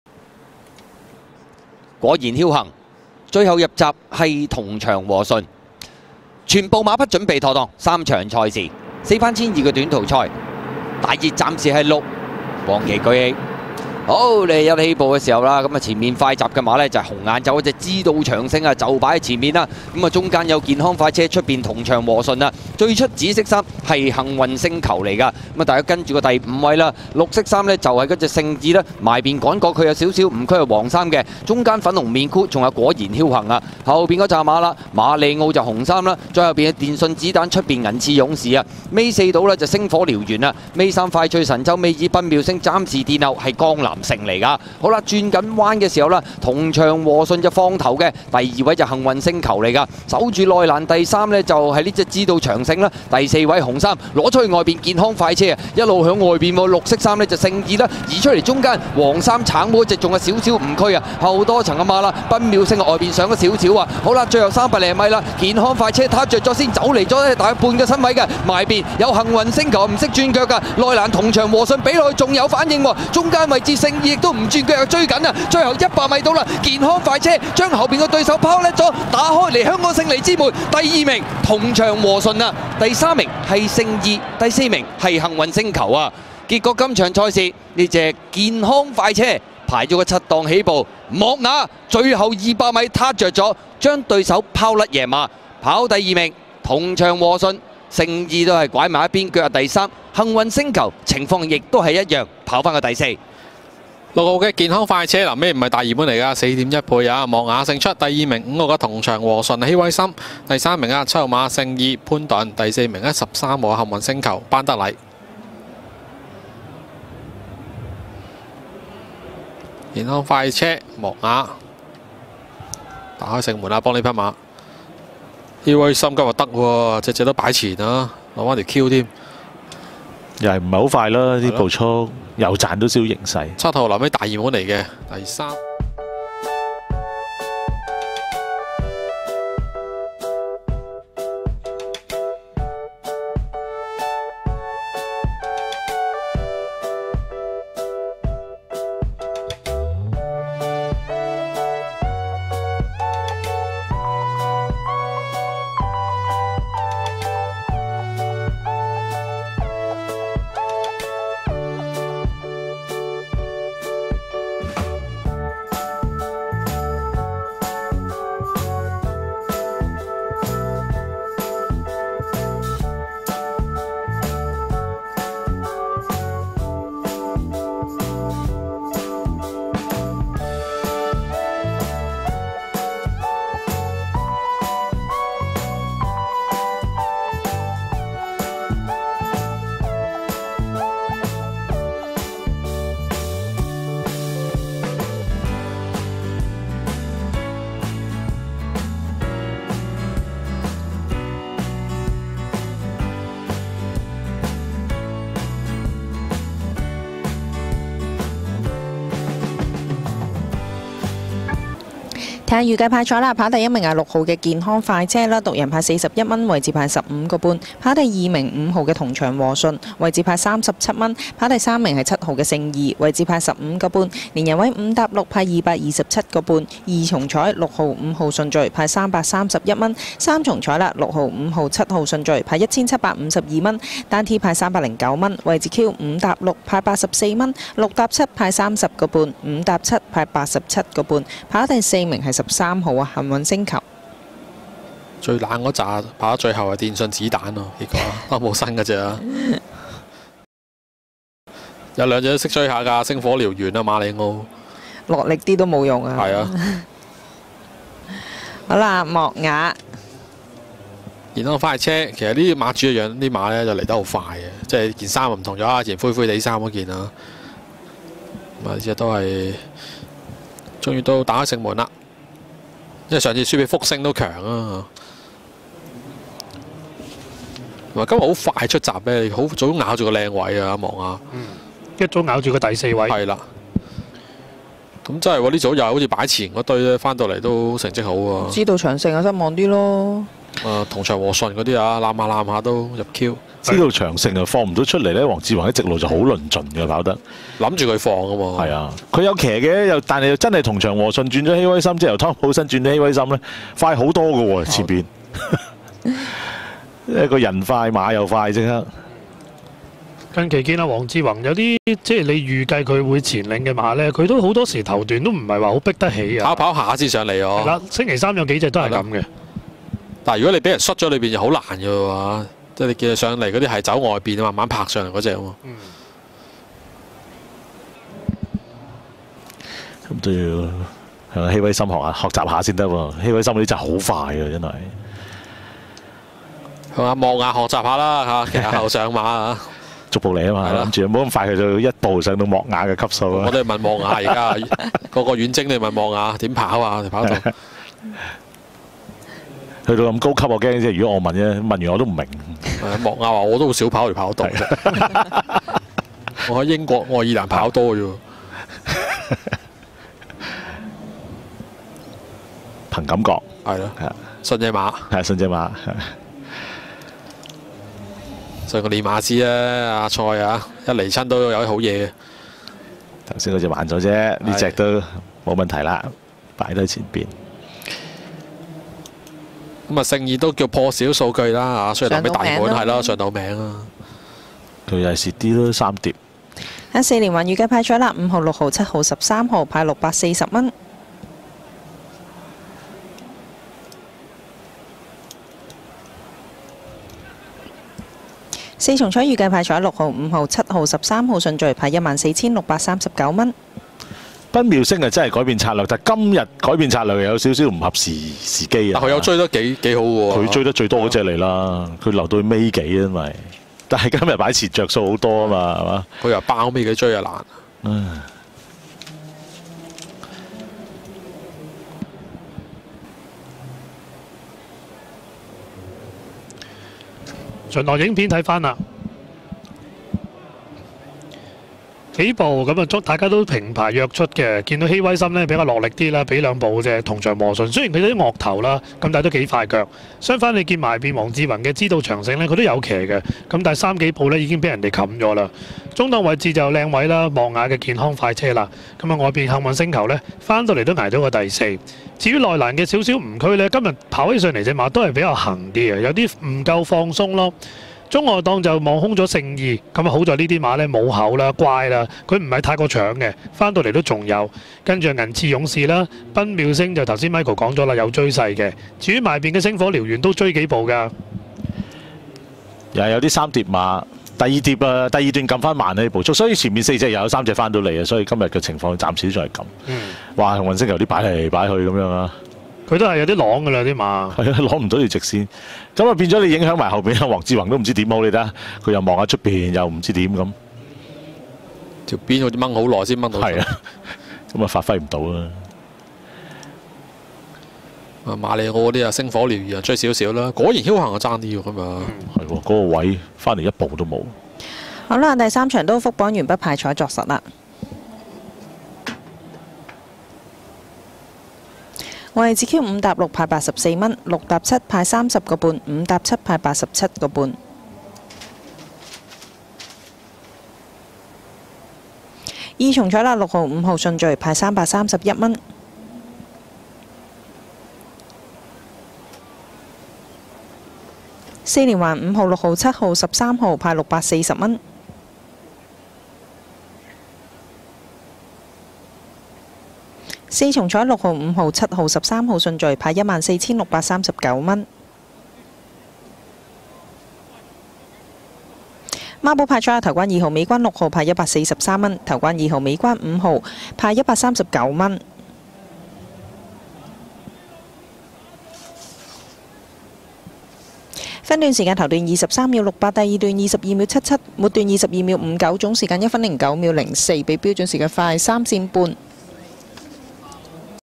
2023/24 (524) - LifeLine Express (健康快車) - 莫雅 (強配贏馬開齋)
Jockey: R Moore (莫雅)
Trainer: John Size (蔡約翰)
Owner: Dr Eddy & Nellie Fong (方正博士與方黃吉雯)

2024/03/24 - Hong Kong - Sha Tin (524)

Sire: Shooting To Win
Dam: La Comedienne